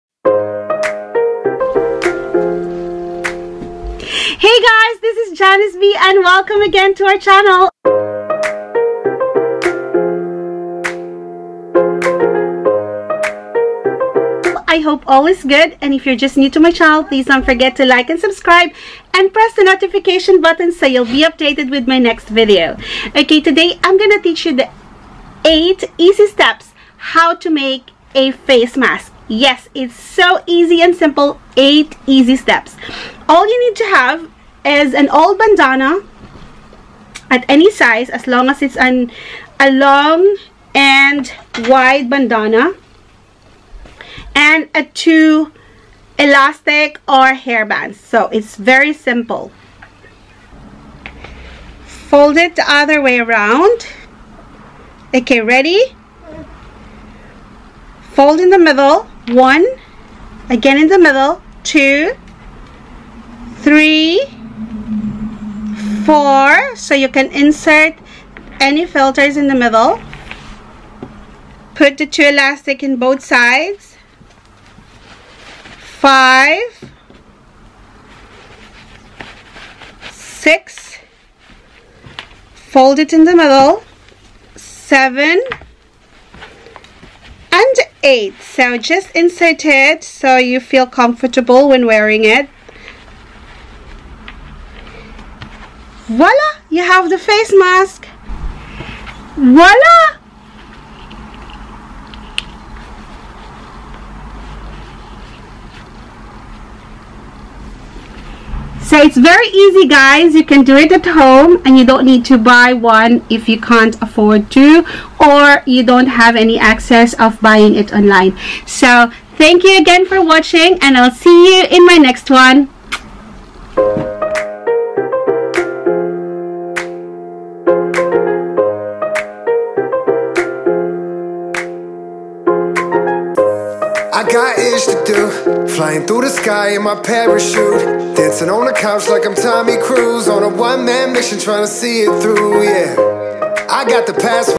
8 EASY STEPS TO MAKE DIY FACE MASK NO SEWING MACHINE | HOW TO MAKE A FACE MASK WITH BANDANA
Do you want to QUIT? or CONTINUE in YOUTUBE?"
As we are all recommended to wear a face mask, here's a very easy way to make a face mask at home. Let's just be creative if we can't buy or have an access of buying a face mask.

Please note that you need to wash your face mask regularly.

To make your own mask using just three things: a bandana, and two hair ties.